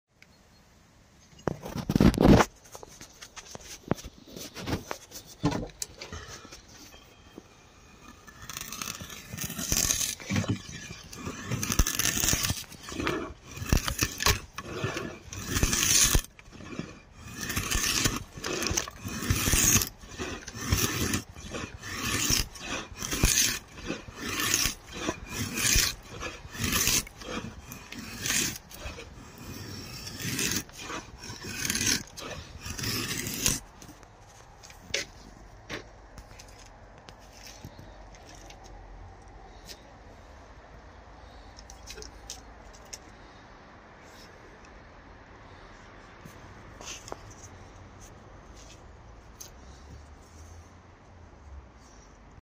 handmade recycled steel draw knife.
Testing forged woodworking tool made from old leaf spring.